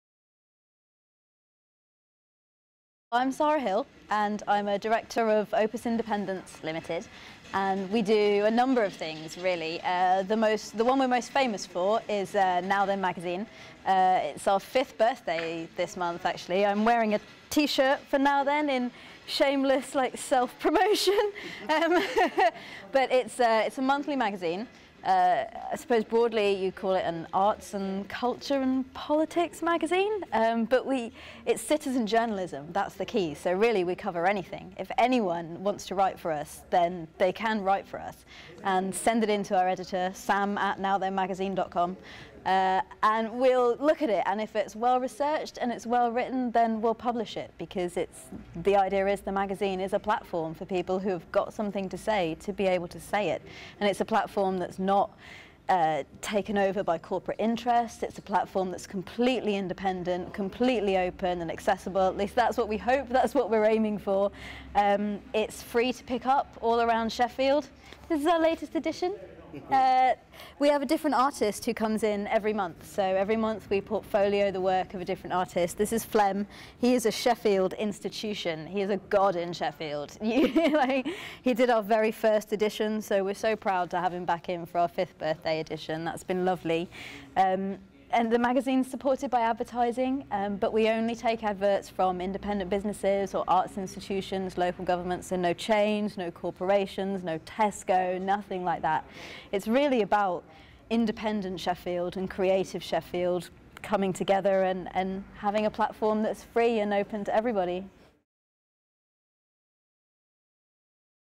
RISE:Sheffield Sara Hill
RISE: Realising & Implementing Social Enterprise, 23rd April 2013. A chance to network with like-minded people creating new digital opportunities as individual entrepreneurs and through Sheffield's network of Digital Media Centres, including:

- The launch of SCN's Internet Access Review
- Practical advice and support with surgeries on finance and business planning
- Promote your business with a 60 second promo video
- Workshop on how to evaluate your impact and prove your worth
- Free lunch!